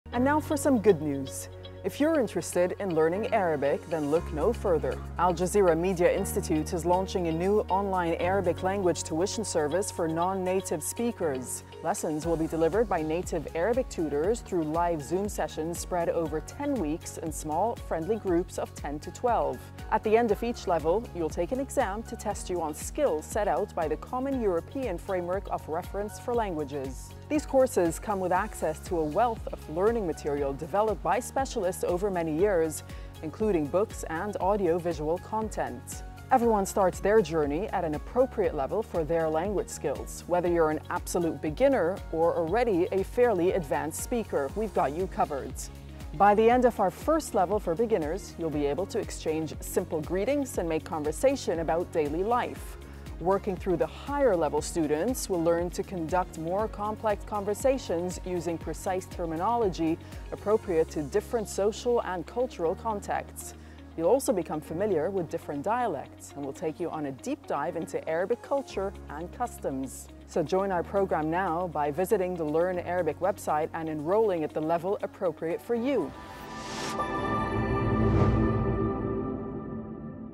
Your Journey to Arabic Proficiency Starts Here!
Learn Arabic the Al Jazeera Way! Join our online courses for non-native speakers, led by expert native tutors in live Zoom sessions over 10 weeks. Test your skills, access rich materials, and explore Arabic culture.